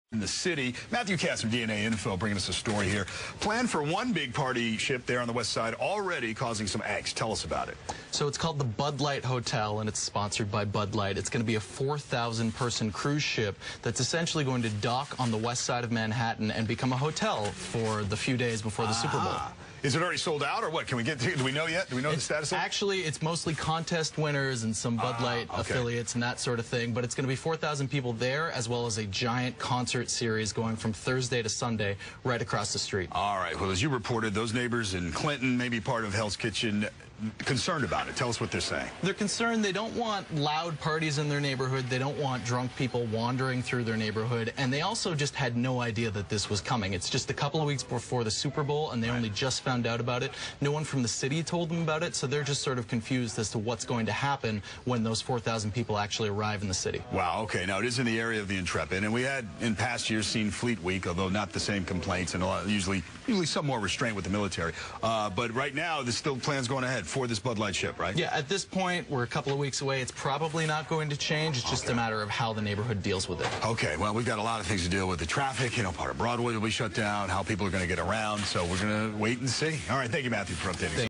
DNAinfo report: Bud Light Cruise Ship to Dock in Hudson for Super Bowl, Brewing Resentment
DNAinfo.com New York's Mathew Katz talks with WNBC's David Ushery about plans for a Bud Light cruise ship to dock in the Hudson River